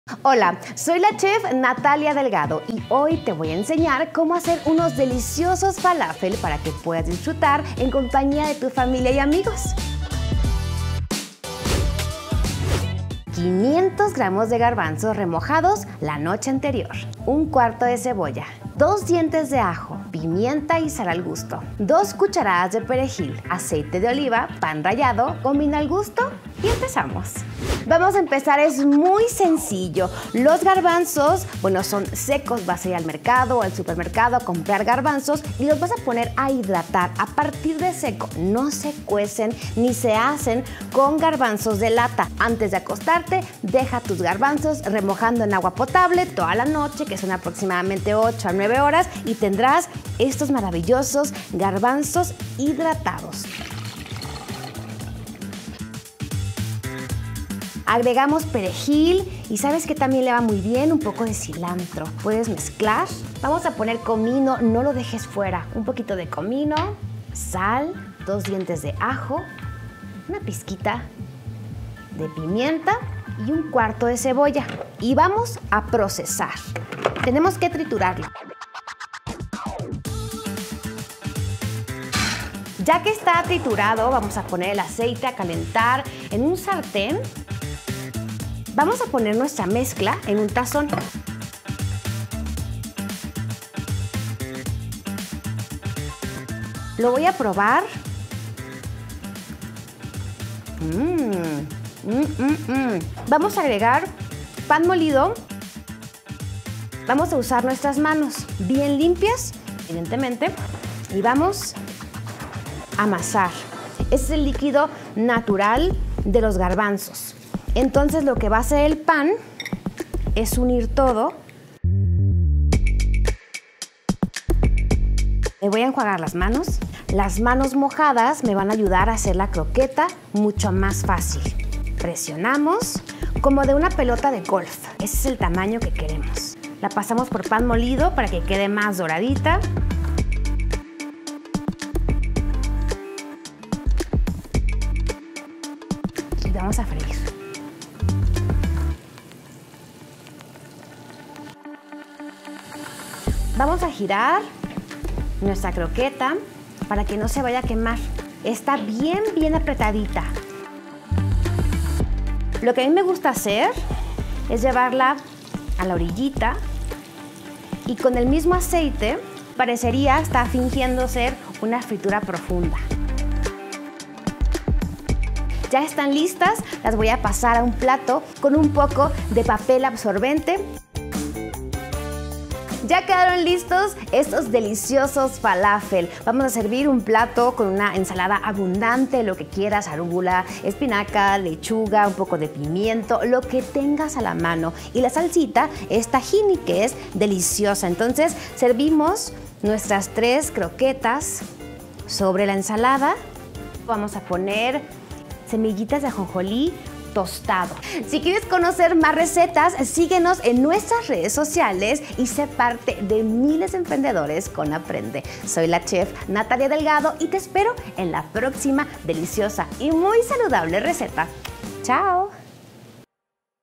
Faláfel fácil con la Chef Natalia Delgado | Aprende Institute
Descubre cómo preparar faláfeles en poco tiempo con la chef Natalia Delgado. Estas deliciosas croquetas de garbanzo son saludables y fáciles. Disfrútalas con toda la familia.

Conoce la receta completa paso a paso 👇👇
0:13 Ingredientes
0:28 Preparación